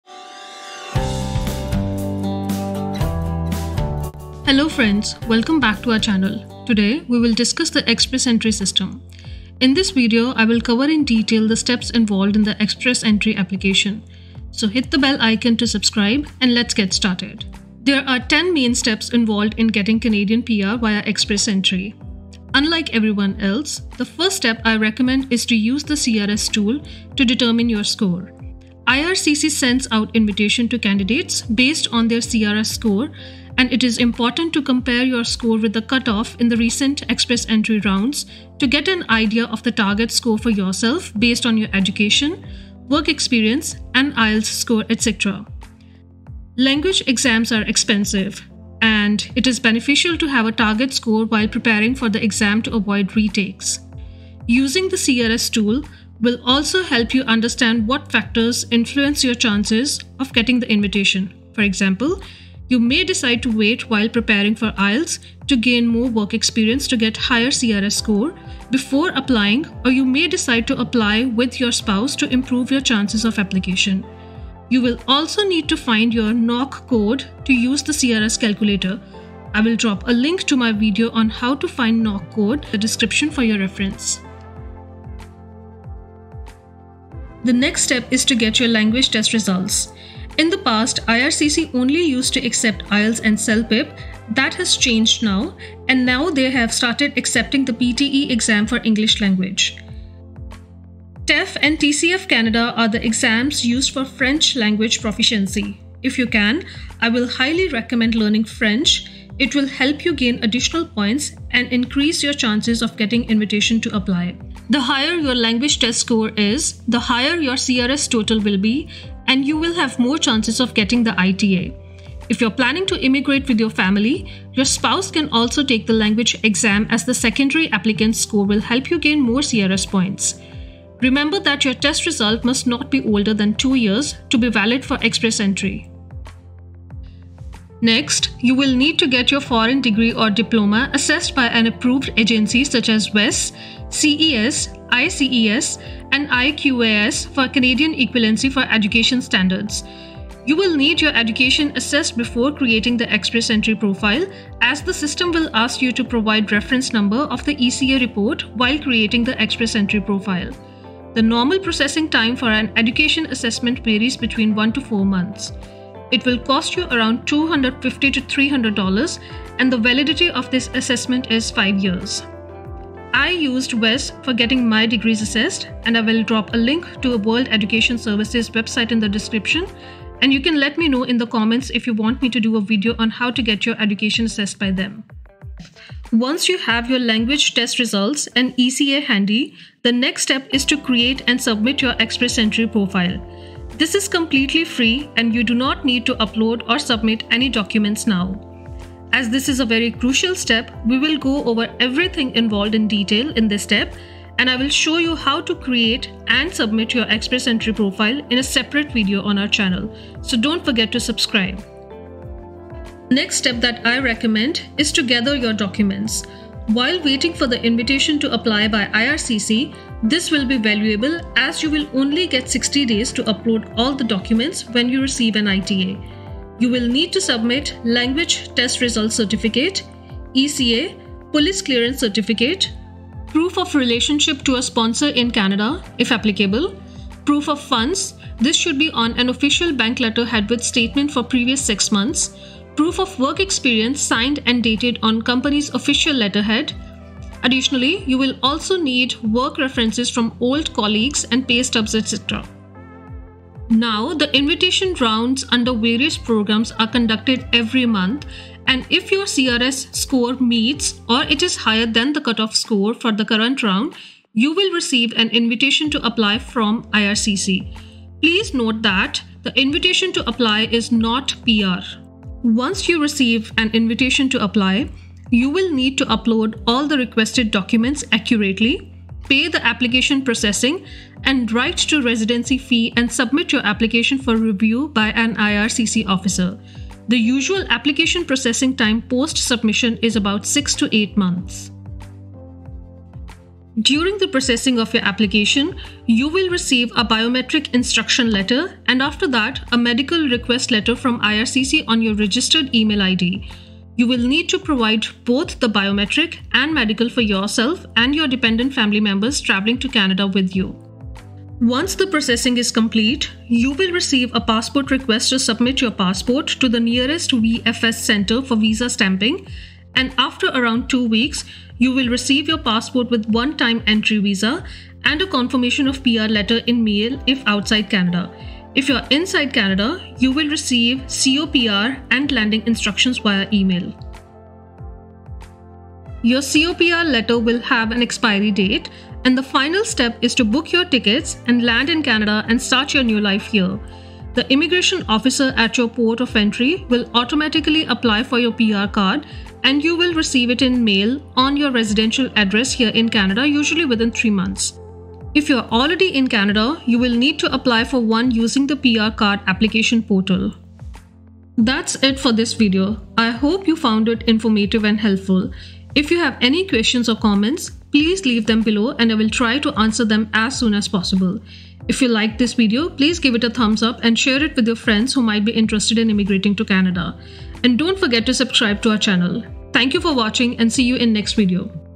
Express Entry: Step-by-Step Guide | Canada PR | Express entry Profile | Express Entry Process 2024
🍁 Welcome to our comprehensive guide on applying for Express Entry to Canada! 🇨🇦 in 2024. In this video, we'll walk you through the entire process, from determining your eligibility to submitting your application and receiving your Invitation to Apply (ITA).

*Timestamps :*
0:00 Intro
0:18 Steps involved in Express Entry
0:24 Step 1: Calculate your CRS Score
1:27 Step 2: Take Language Exam
2:17 Step 3: Educational Credential Assessment (ECA)
3:09 Step 4: Create and Submit your EE Profile
3:32 Step 5: Gather your Documents
4:14 Step 6: Get an Invitation to Apply (ITA)
4:32 Step 7: Submit your Application
4:51 Step 8: Biometrics and Medical
5:11 Step 9: Passport Request for Visa Stamping
5:37 Step 10: Landing in Canada
6:00 Outro

*Resources Mentioned:*

💬 Got questions or need assistance? See you in Canada! 🇨🇦🥳